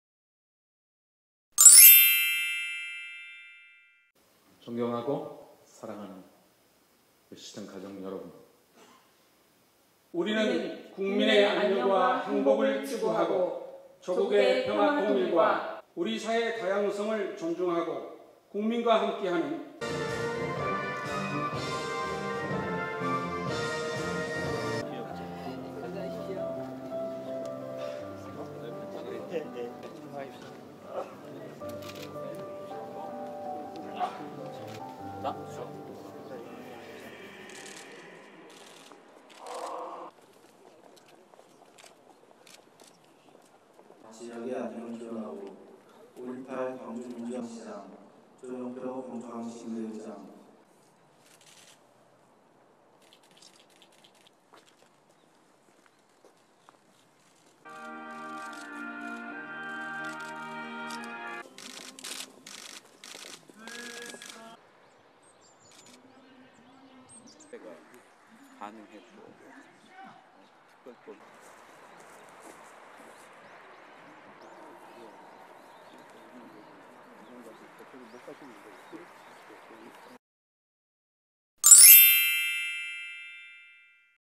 광주광역시 2016년 시무식
윤장현 광주광역시장은 4일 오전 시청 3층 대회의실에서 열린 2016년 시무식에 참석해 후손들에게 물려줄 넉넉하고 따뜻하며 당당한 광주 건설을 위해 올 한해도 공직자들의 열정과 헌신을 당부했습니다.

또 윤시장은 4일 오전 조영표 시의회 의장, 시 간부, 공사공단, 출자 출연기관장 등과 함께 광주독립운동기념탑, 현충탑, 4·19탑, 학생독립운동기념탑, 국립5·18묘지를 찾아 신년맞이 합동 참배했습니다.

영상제공 : 광주광역시
편집기자 : 박범순